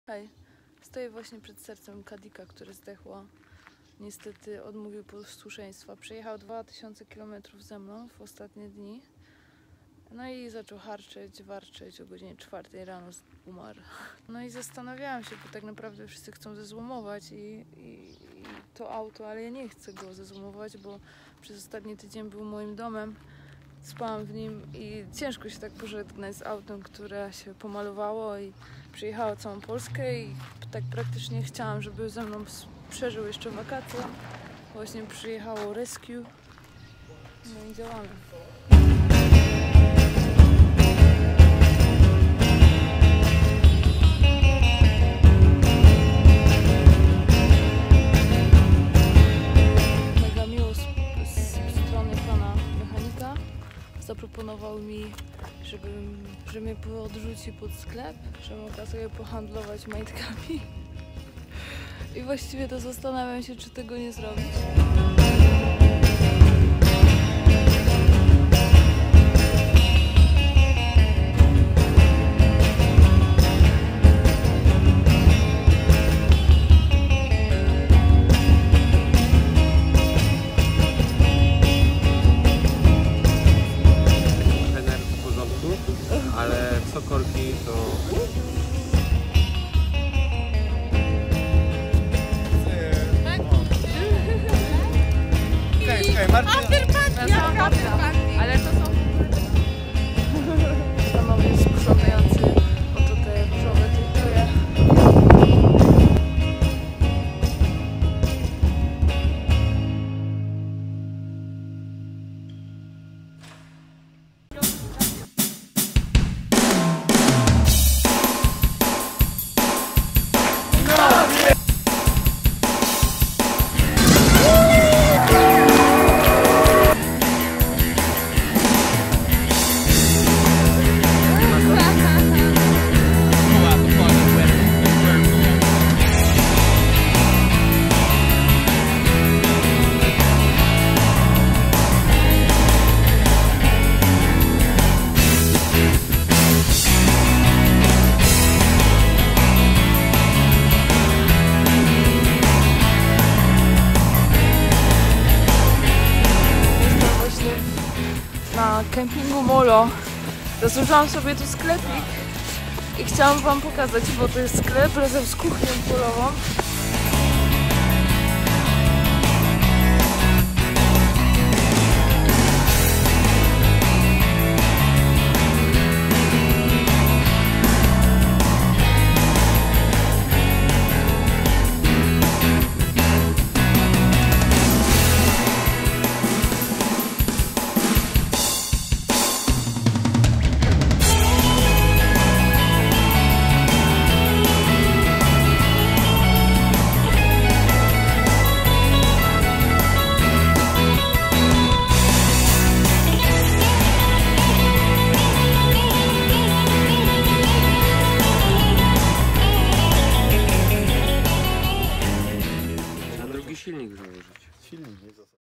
Majtko-wozem przez świat odc.3 - piątek 13-stego oraz wycieczka nad morze
Nigdy nie wierzyłam w pechową aurę piątku 13-stego, aż do czasu,, kiedy serce mojego ukochanego majtko-wozu przestało bić!
Ponad rok temu, uliczna instalacja sklepu z majtkami, przerodziła się w firmę oferującą śmieszne i oryginalne Majtki z Sosnowca. Jednak czegoś brakowało mi samej sprzedaży przez Internet. Chciałam mieć żywy kontakt z klientem, tak by mógł uśmiechnąć się na widok różowego VW Caddy z 1998 roku. Auto zakupiłam kilka miesięcy temu za 2K i już następnego dnia wszystko w nim się zepsuło, włożyłam więc w niego raz tyle. Spędziłam tygodnie malując go i renowując, tak by mógł ze mną przeżyć wakacyjną przygodę! Na początku lipca wyruszyłam ze Śląska w stronę pomorza, zachaczając o ul.Starowiejską 40 w Gdyni, gdzie poznałam niezwykłych ludzi z Honolulu oraz Czerwony Piec. Z początku nie planowałam pobytu na Openerze (Nie chcieli naszych czystych majtek, pozdrawiamy też Sloggi), ale spróbowałam rozłożyć stragan na parkingu, aż w końcu nabyłam też bilet. Po przepięknych dniach w 3city, ruszyliśmy na Hel, spotykając starych znajomych, próbując surfingu, sprzedając majtki i świetnie się bawiąc.
Podczas podróży do rodzinnego domu, 260km od celu nagle coś kilka razy stuknęło i Caddik stracił moc. Niestety moje super assistance premium przestało działać i lawetę zamówiłam na własny koszt. Wylądowałam w warsztacie pomiędzy polami i usłyszałam najgorszą diagnozę. Silnik na hasiok٭, a wstawienie nowego to 2K. Właściwie to przed propozycją wymiany usłyszałam, że najlepiej będzie go zezłomować, ale ja nie mogę się tak łatwo poddać! Przed nami jeszcze wiele przygód, podczas których jest on moim ukochanym domem oraz sklepem na kółkach. Pomóż nam kontynuować wspólną przygodę!
Powstał też vlog z naszej podróży, ale nie sądziłam, że zacznę go od końca. Zobacz jak spędziliśmy ubiegły tydzień! Jeżeli podoba Ci się to co robimy i chciałbyś nam pomóc, choćby poprzez udostępnienie tego posta będziemy mega szczęśliwi. A tych z Was, którzy chcieli by się dołożyć finansowo zapraszamy na Polak Potrafi! Dla każdego czyste majtki! No to jak, pomożecie?
 ٭śmietnik